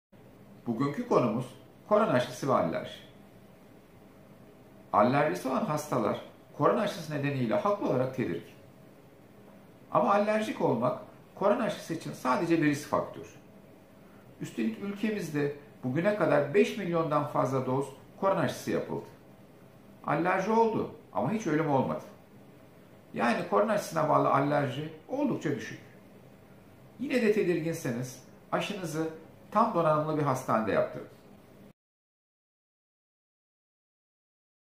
Corona aşısı ve Allerji
Corona aşısına bağlı öldürücü allerji nadirdir. Allerji riskiniz yüksekse hastanede yaptırınız.
Küçük dokunuşlar büyük yararlar sağlayabilir. Bu filmde/görselde genel bilgiler yer almaktadır, doktora danışmadan tedavide değişiklik yapmak sakıncalı olabilir. kitap iyikitansiyonumçıktı iyikiyaşalıyorum kiloverme #37. Akşam yemeği bilgi hayat kurtarır